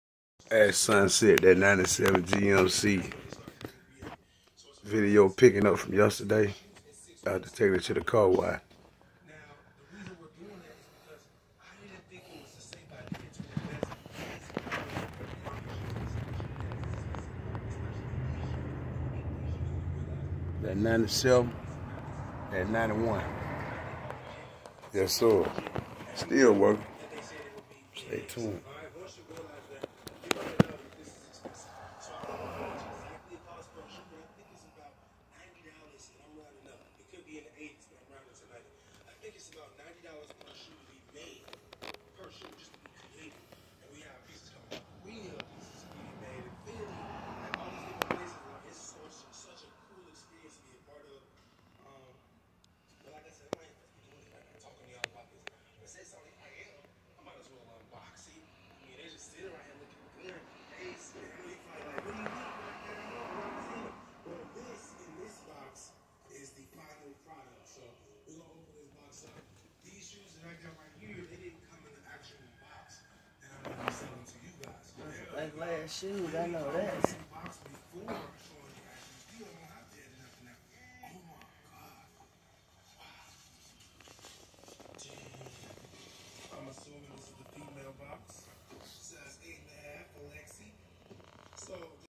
check out the video leaving the car wash cleaning all of the paint dust off 😂
check out the video yesterday today is that a 97 GMC is looking after that car wash getting all the paint dust off of it 😋